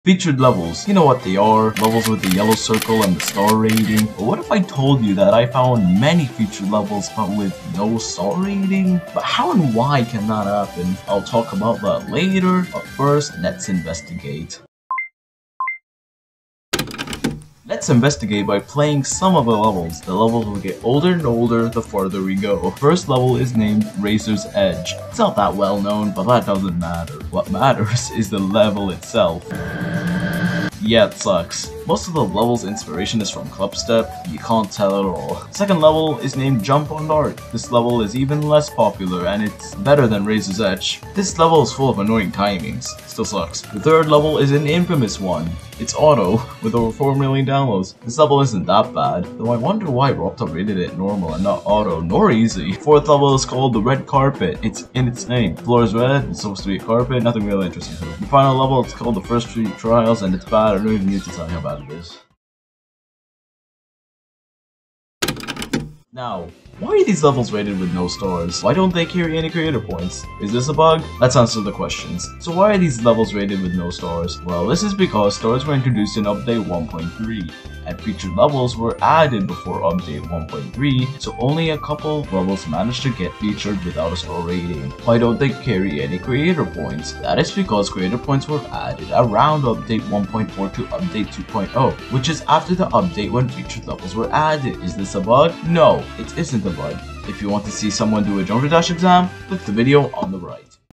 These Featured Levels Have NO Stars?
in this video, i went through and found many levels that were featured, but with no stars? i went through and may have found the reason on why these kind of levels exist

im sorry this is short! i couldn't get enough time to work on this for a long time, tho ill make a promise, next commentary video will be a banger and a long video, trust me!